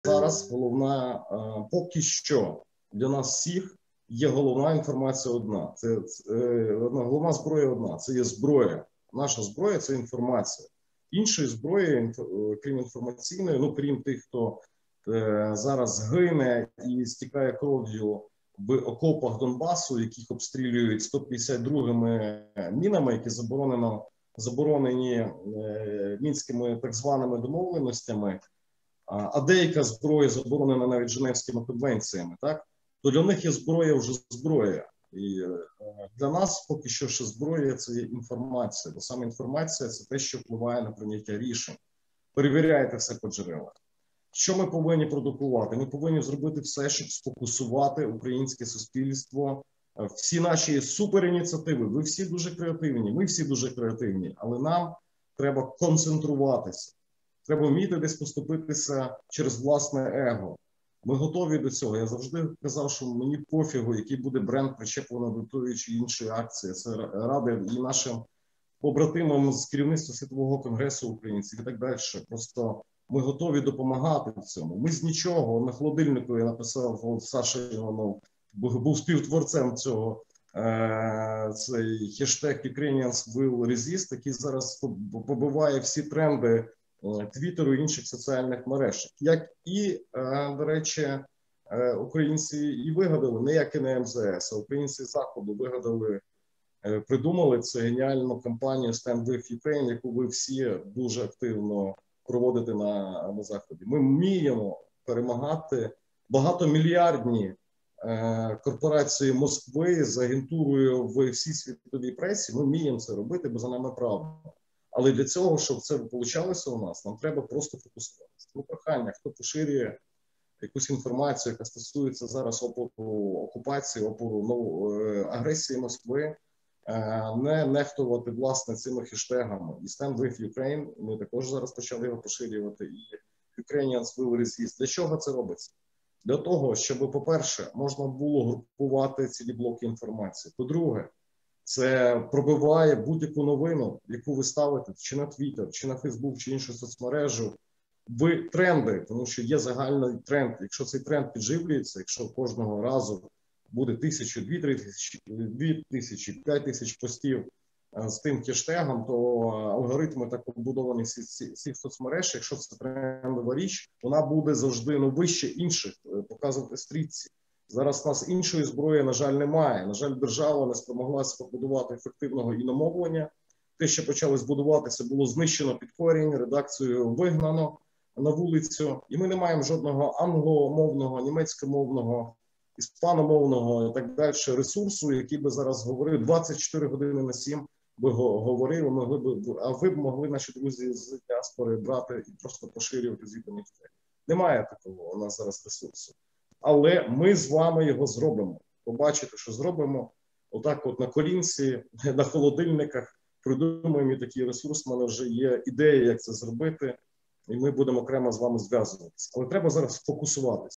Андрій Левус: Наша головна зброя це інформація
✊Рух опору капітуляції ініціює створення Міжнародного громадського комітету з притягнення до кримінальної відповідальності Путіна та представників його режиму.